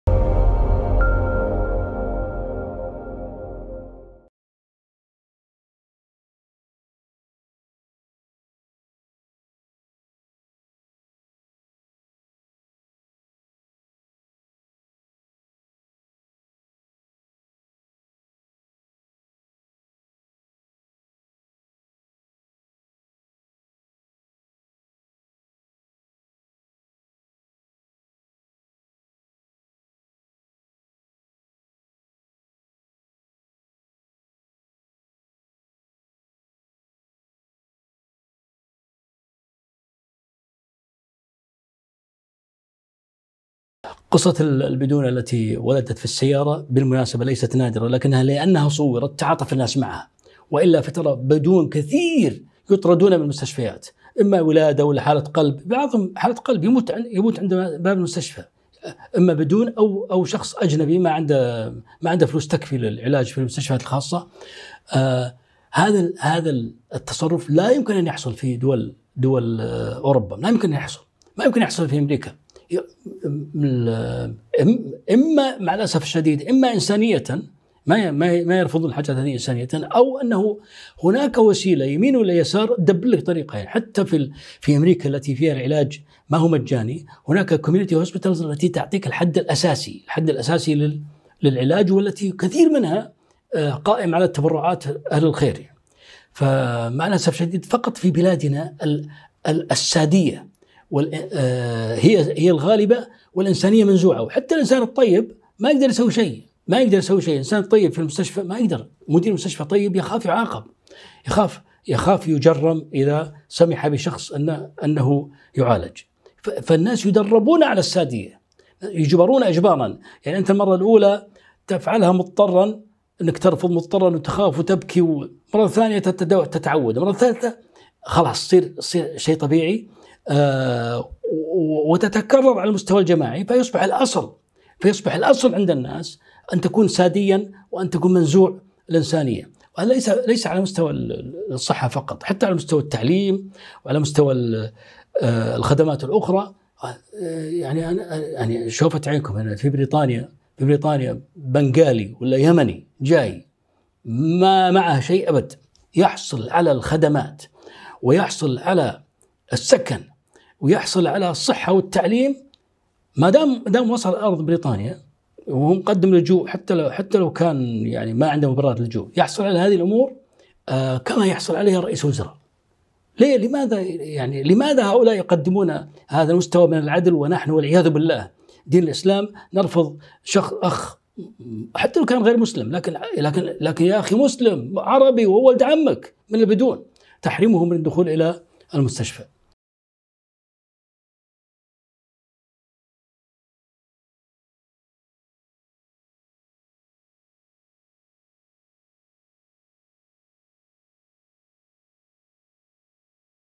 The BIDOUN / BIDUN / BIDOON: An Underclass In Gulf Countries
The fake 'islamic' regimes of the Gulf have been birthed by the British and the French colonialists in the aftermath of the Ottomans' decline. These dictatorships have a consistent policy of dividing their own captive populations into a caste system, and the Bidouns / Biduns / Bidoons are at the bottom of it, along with low-wage foreign workers. These 'Muslims' sadistically enjoy torturing those lower classes.
Dr. Sa'ad Al-Faqih explains why.